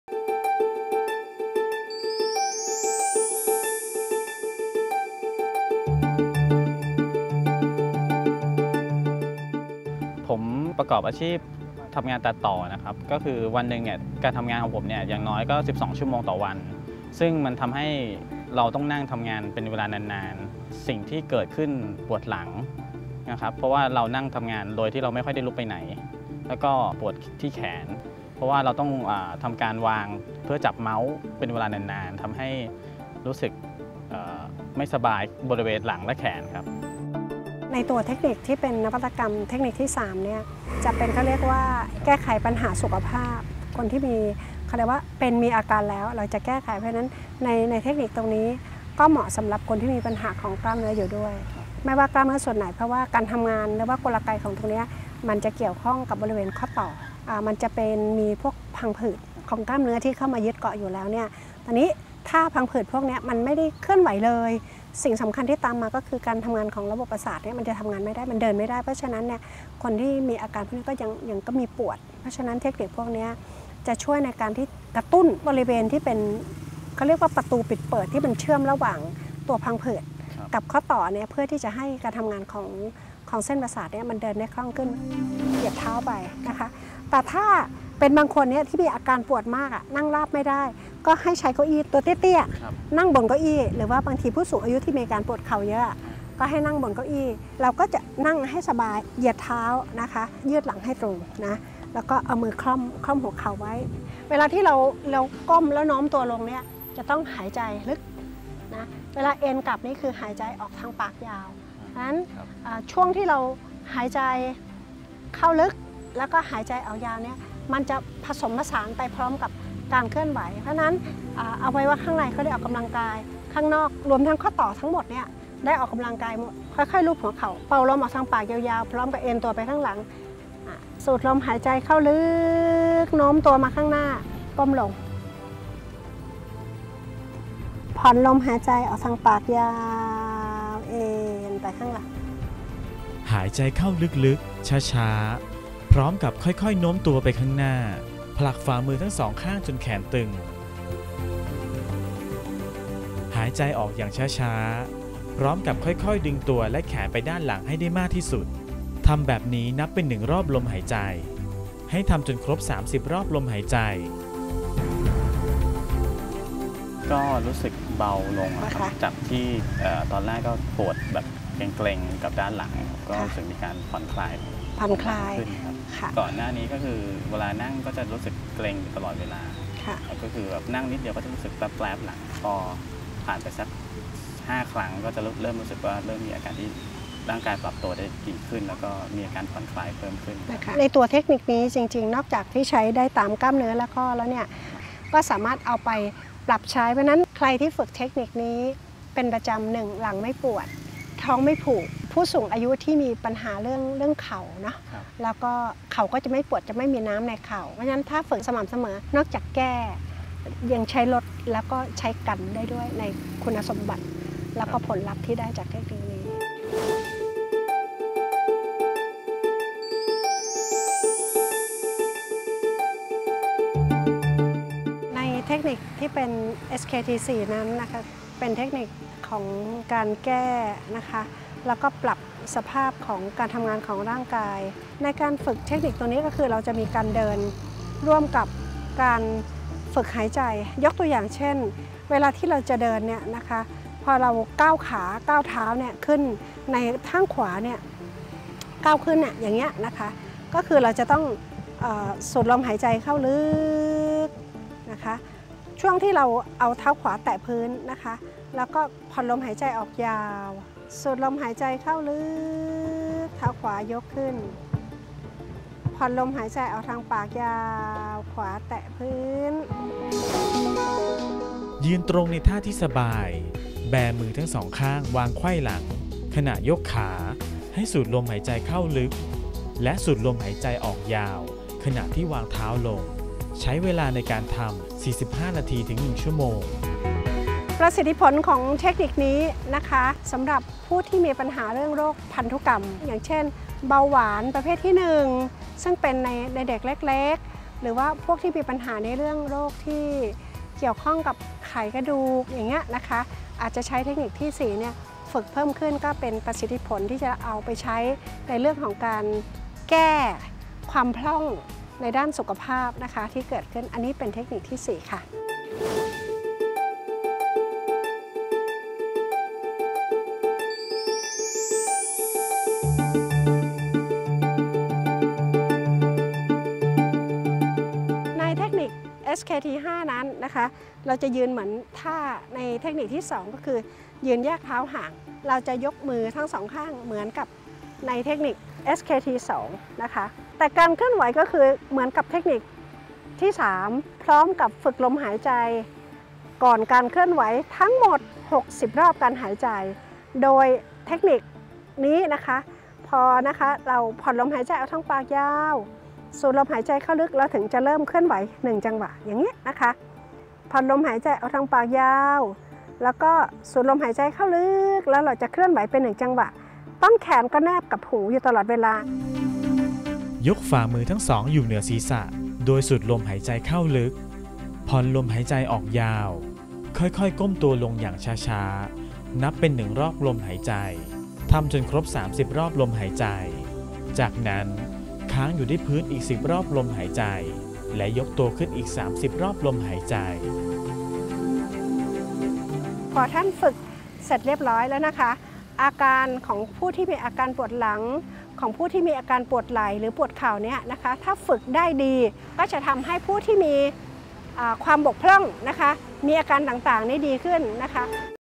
สมาธิบำบัดแบบ SKT ท่าที่ 3 - 5 ปรับสภาพร่างกาย ออฟฟิศซินโดรม ปวดหลัง ปวดเข่า ปวดข้อ [by Mahidol]
สมาธิบำบัดแบบ SKT ท่าที่ 3 ปรับสภาพร่างกาย  สมาธิบำบัดแบบ SKT ท่าที่ 4  ช่วยบำบัดโรคทางพันธุกรรม เบาหวาน คนที่ปัญหาเกี่ยวกับไขกระดูก สมาธิบำบัดแบบ SKT ท่าที่ 5 ปรับสภาพร่างกาย ให้ผู้ป่วยที่มีอาการปวดหลัง ปวดไหล่ ปวดเข่า สมาธิบำบัด SKT เพื่อบำบัด และเยียวยาผู้ป่วยโรคต่างๆ จนพัฒนารูปแบบสมาธิบำบัดออกมา คิดค้นโดยรศ.ดร.สมพร กันทรดุษฎี เตรียมชัยศรี เมื่อเป็นอาจารย์ประจำภาควิชาอาชีวอนามัยและความปลอดภัย คณะสาธารณสุขศาสตร์ มหาวิทยาลัย มหิดล

Change [by Mahidol] สมาธิบำบัด SKT ทางเลือกใหม่ไร้โรค